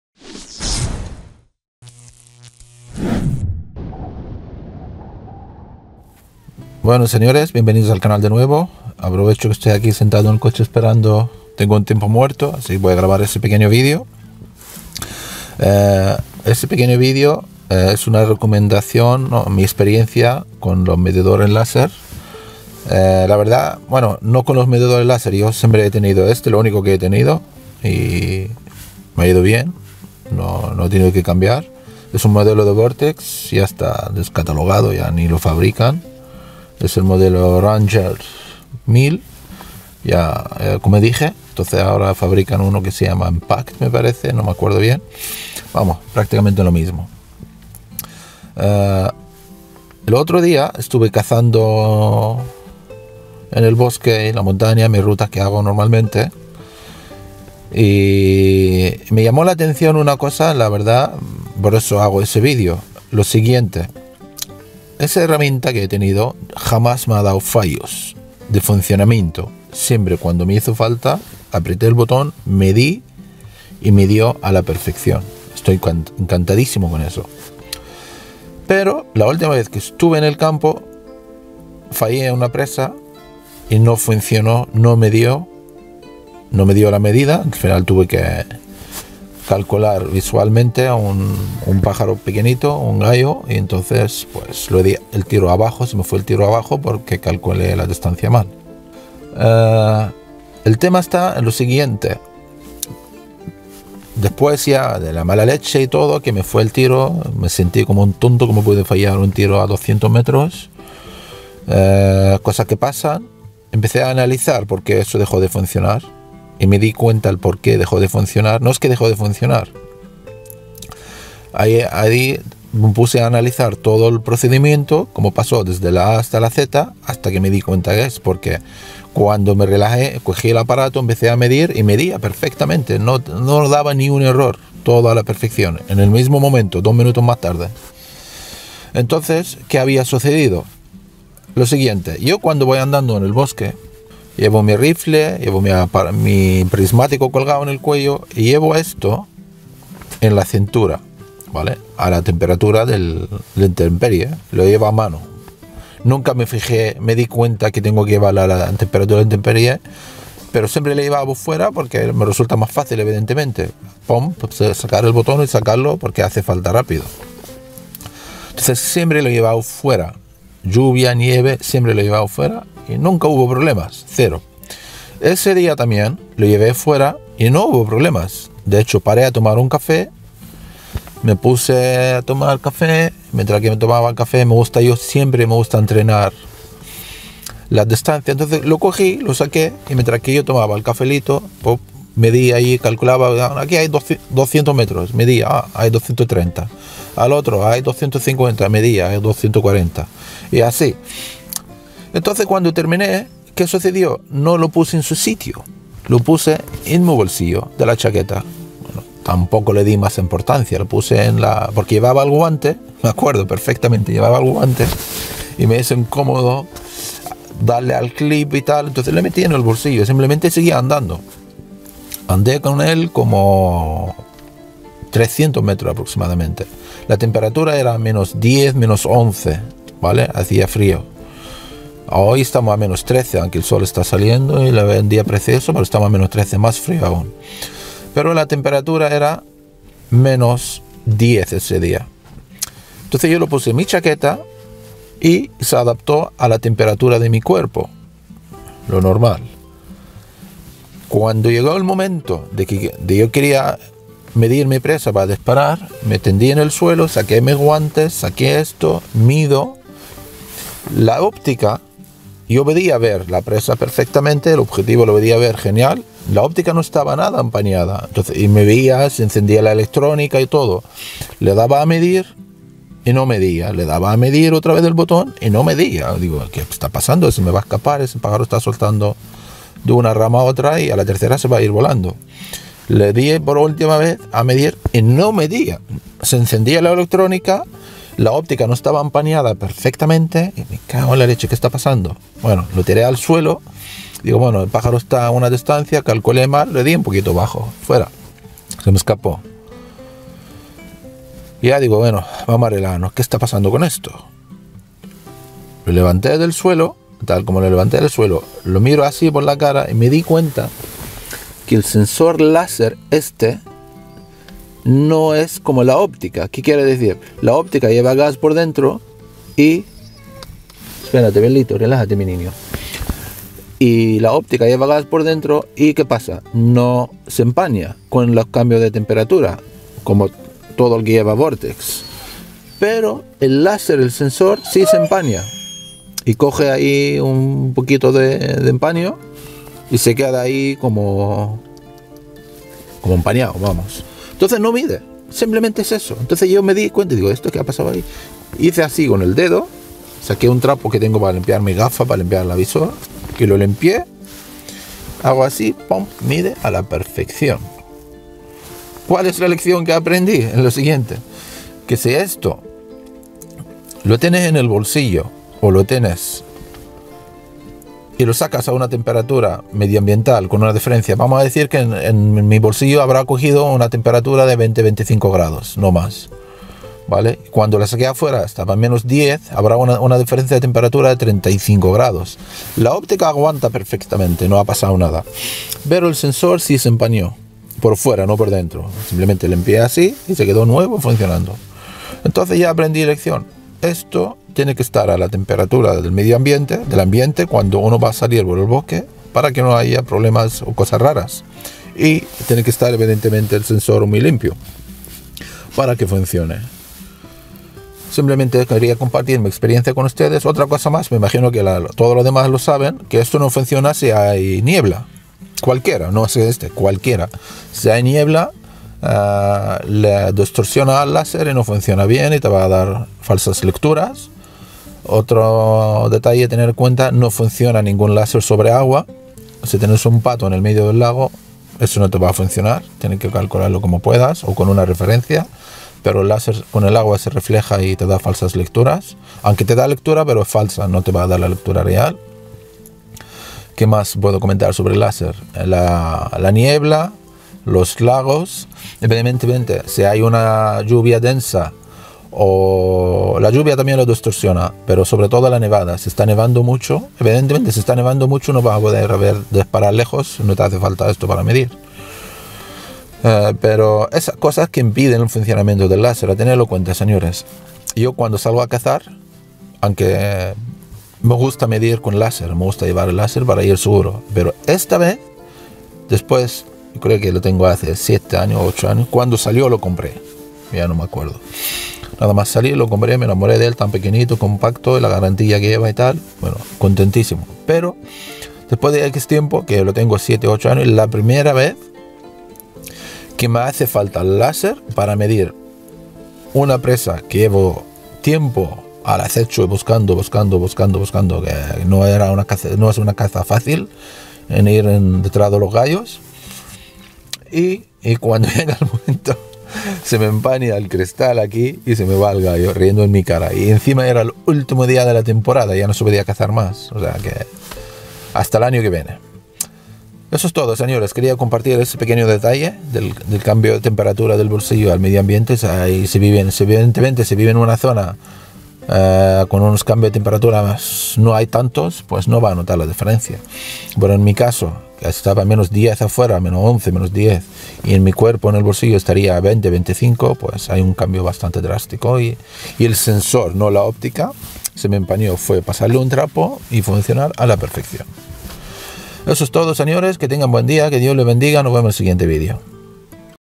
Mi experiencia con el telemetro laser
una anécdota pasada por mi experiencia con el medidor de distancias laser en la última salida de caza, la comparto con ustedes para no caer en el mismo fallo que yo, espero que sea útil